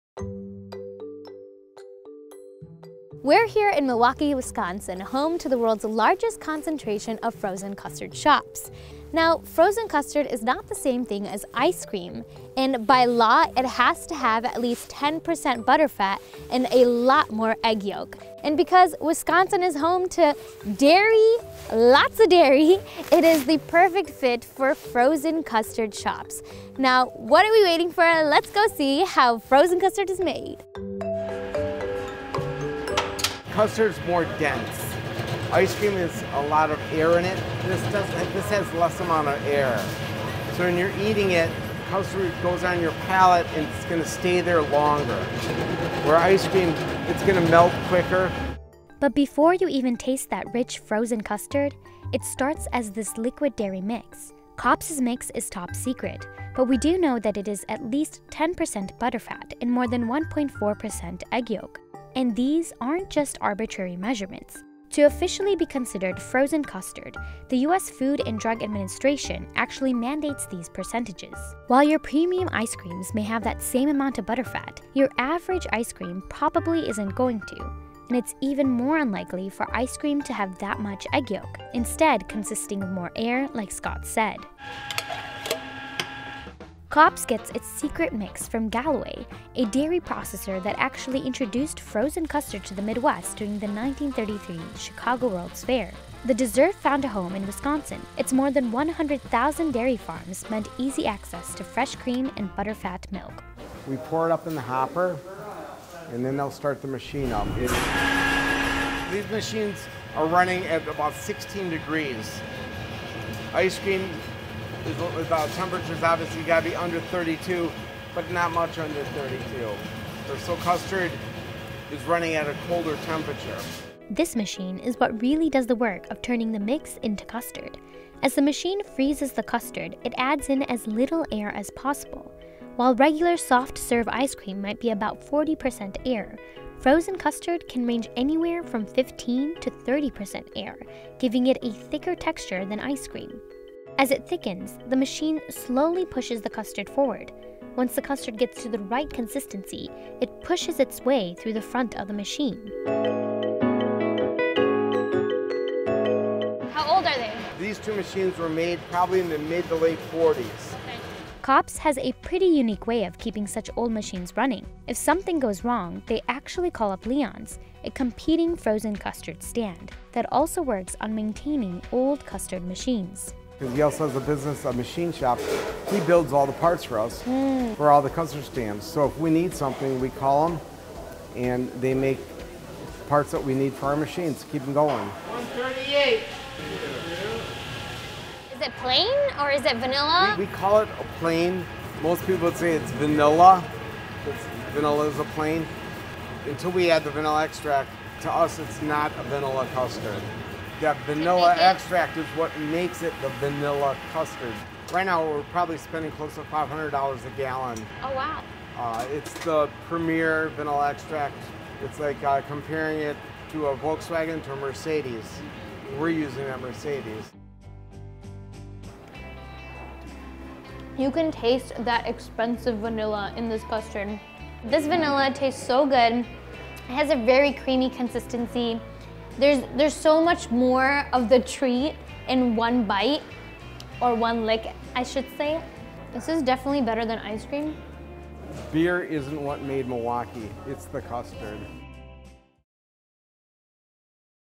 How Frozen Custard Became Wisconsin’s Favorite Dessert | Regional Eats
Frozen custard is usually made with more egg yolk and butterfat than ice cream. Wisconsin's dairy farms make the state an ideal home for the dessert.